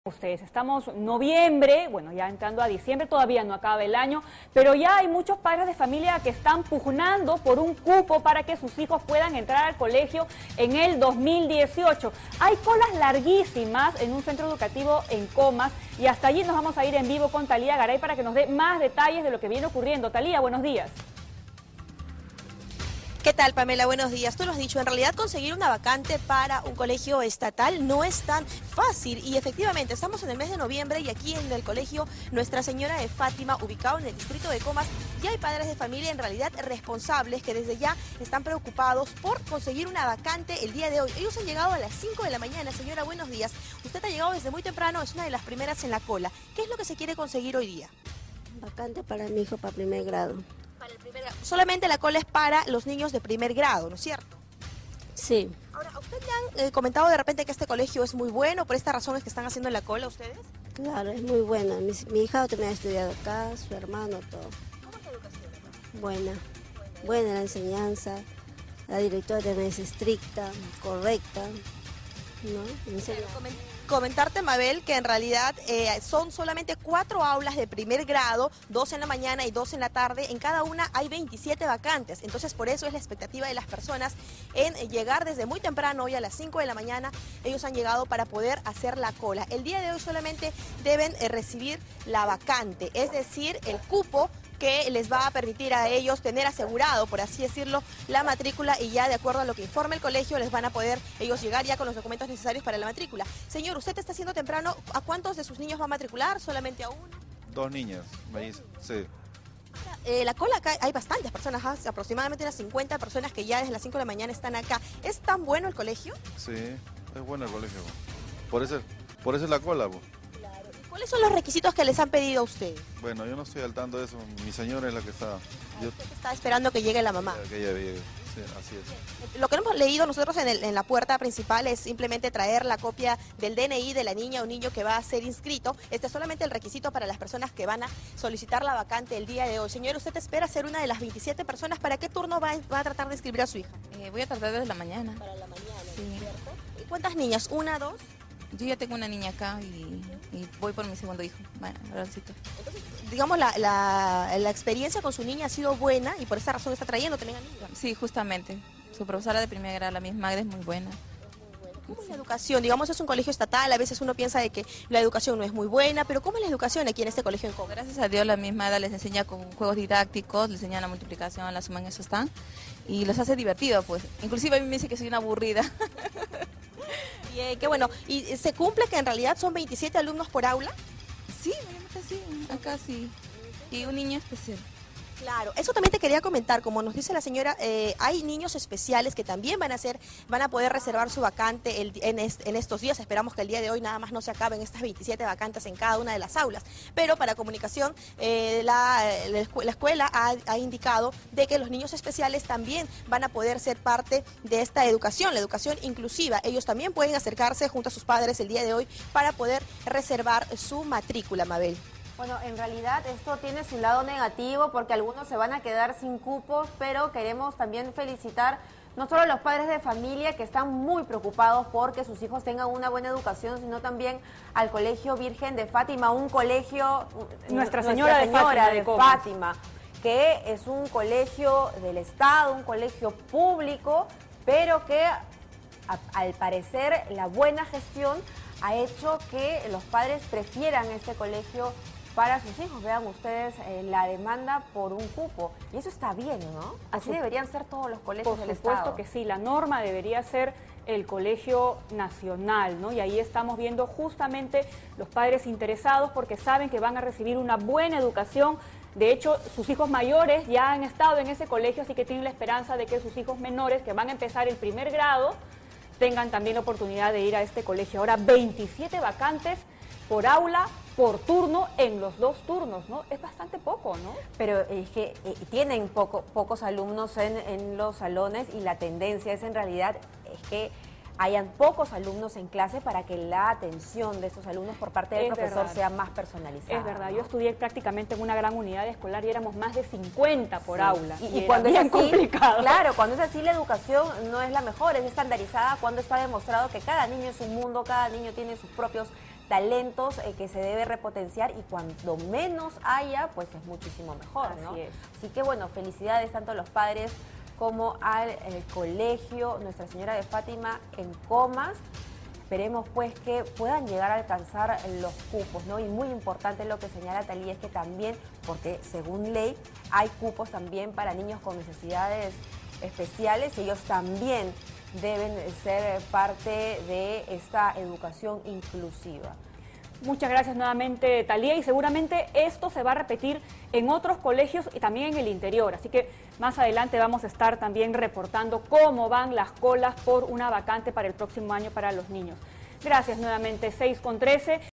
Comas: padres se amanecen haciendo colas para conseguir vacantes en colegio estatal
Decenas de padres de familia realizan largas colas para poder solicitar una vacante de matrícula en el colegio estatal Nuestra Señora de Fátima ubicado en el distrito de Comas. Muchos de ellos se apostaron en los exteriores de la institución desde la 5 de la mañana.